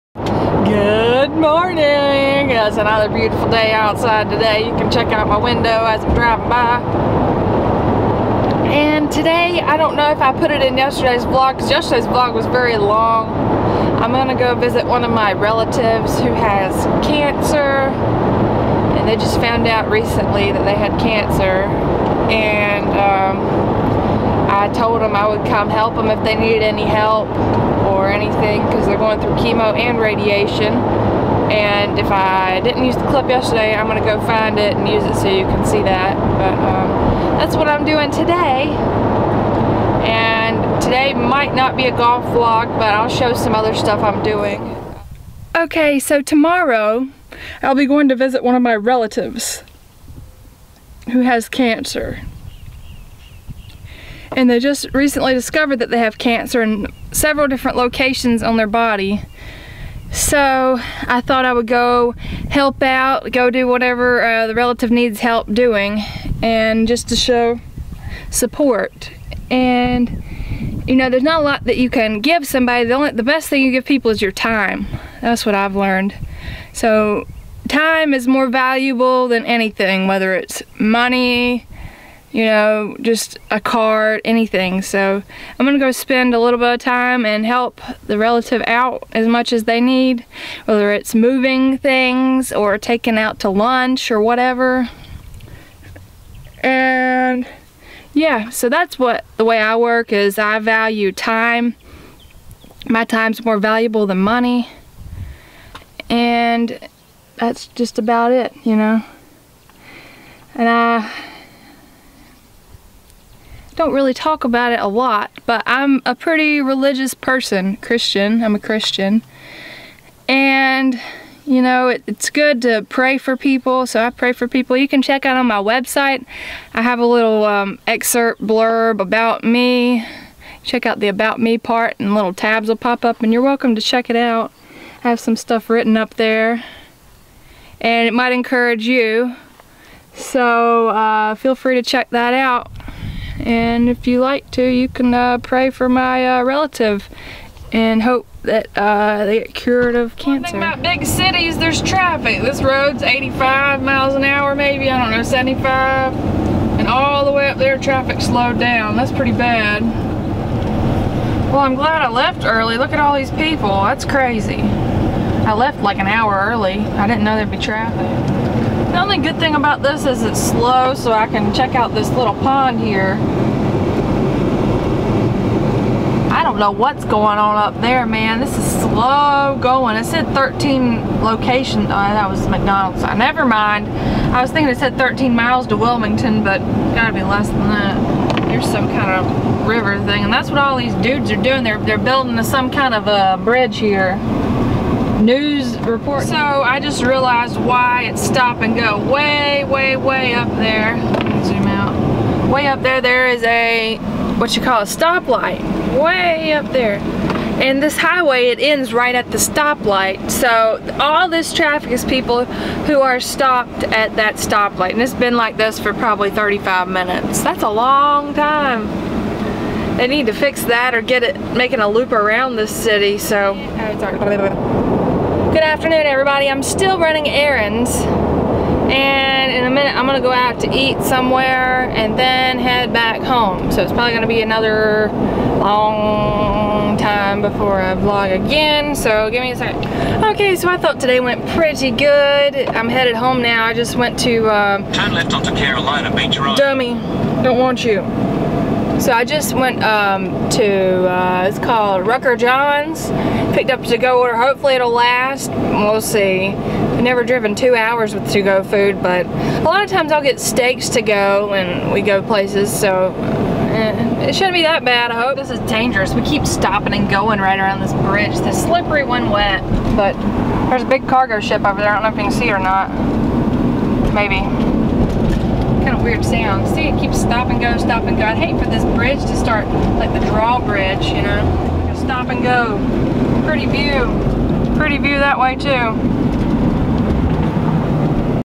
Car Vlog day 201 Me Talking For 5 Min
The title says it.
Busy all day driving. I didnt really have anything i could vlog and make entertaining . So i tried to narrate what was going on with the road system. More golf tomorrow :)